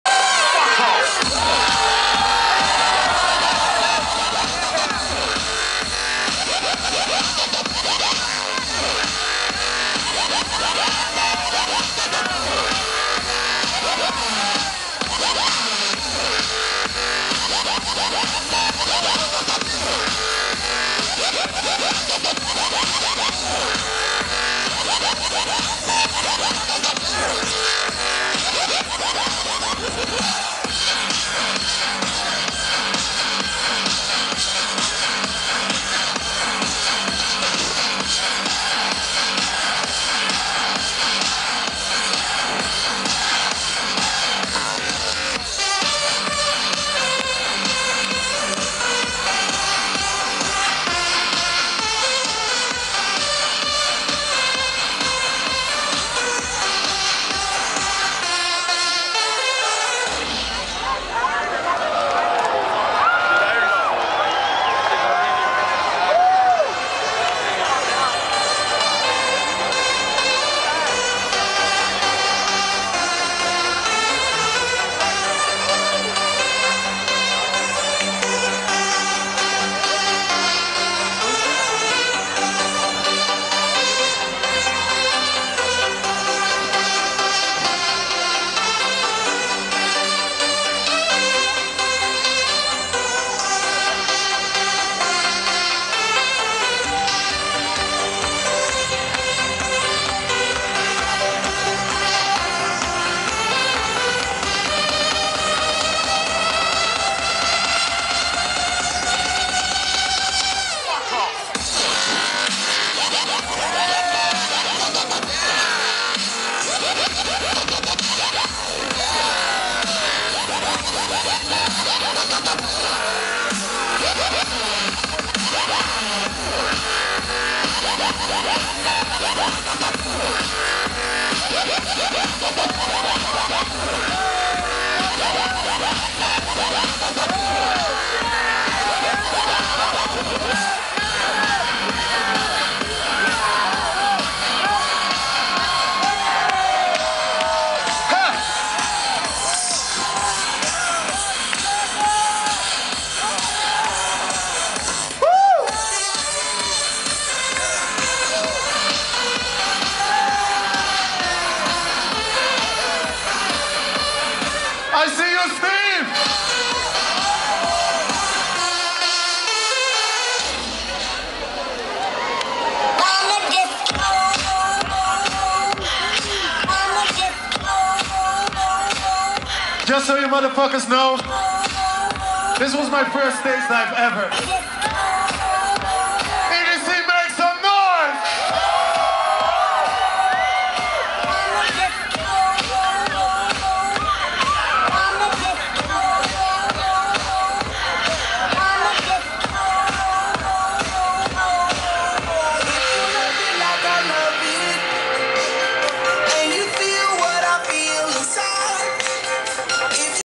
Steve Aoki and Afrojack EDC Las Vegas 2012
Steve Aoki playing with Afrojack during EDC Las Vegas 2012. This was totally insanse, with inflatable rafts being used to body surf! Now that's a wild carnival ride!

ID?

Please contact if the audio, visual or performing artists would like to remove this video for copyright reasons.